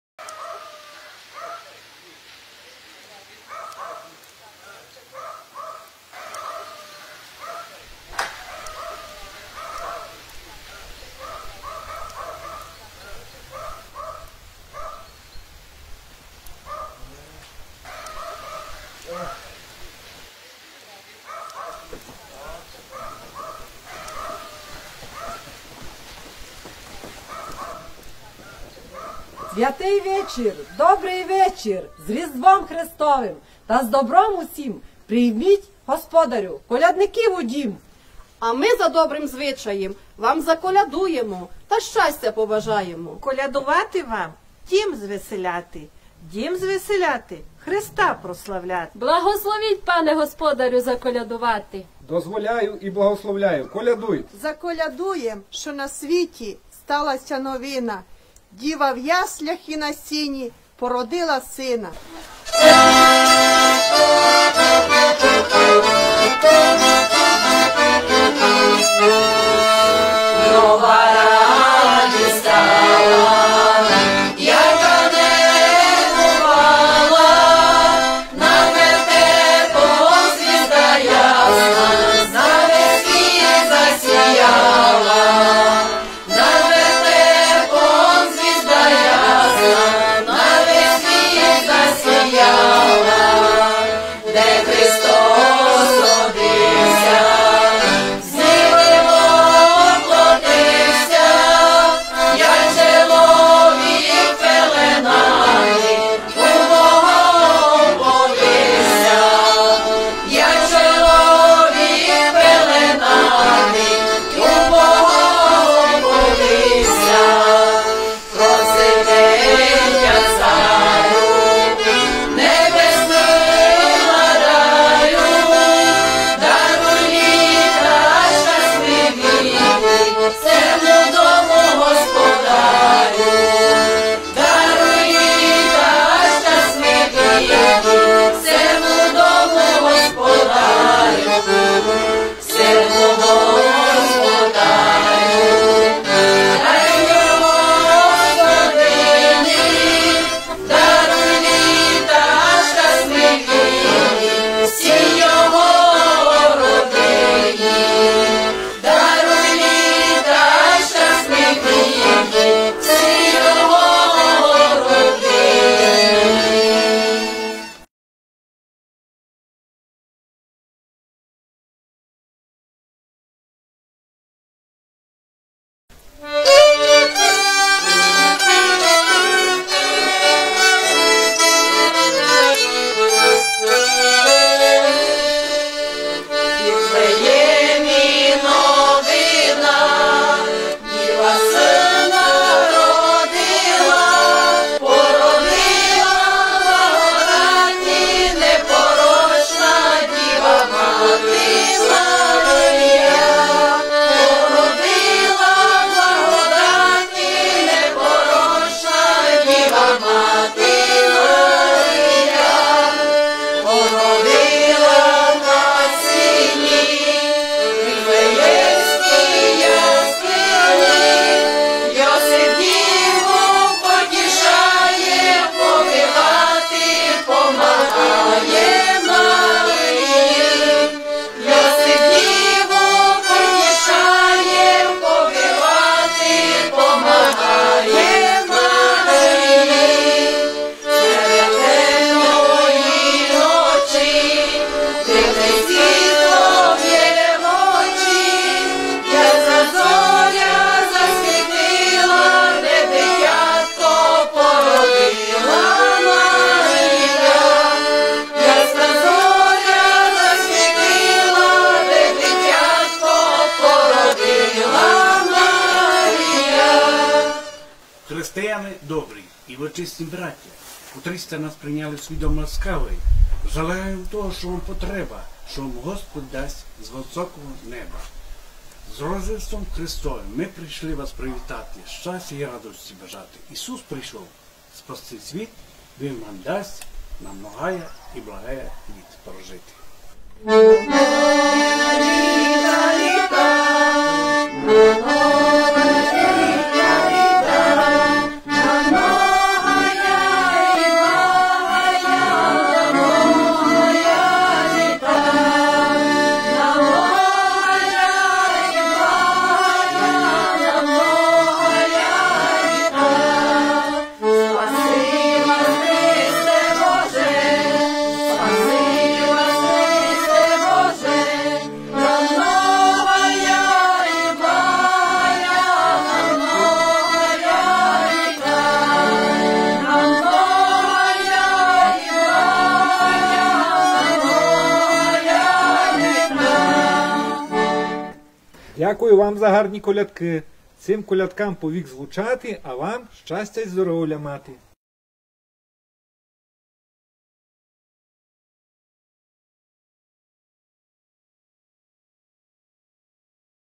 115. Фольклорний колектив "Папороть". Горінчівська ТГ, Хустський район, Закарпатська область.
Створений у лютому 2021 року з клубних та бібліотечних працівників культури Горінчівської ТГ